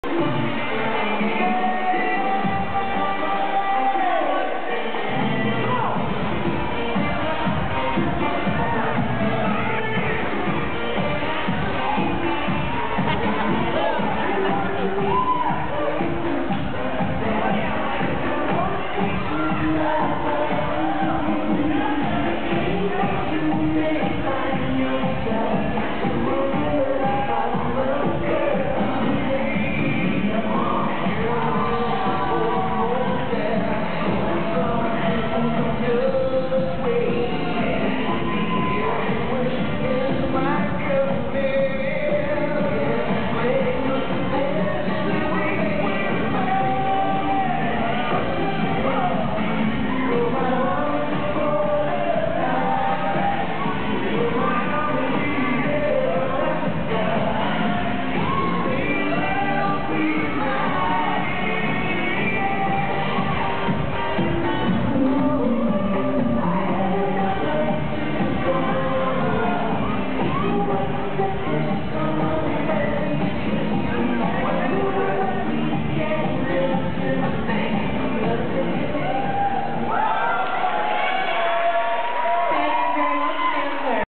2008 UCONN Ballroom Comp - Silver Cha Cha
Ithaca College DanceSport
Katie & Vince dancing Silver American Cha Cha in the CC/Rumba event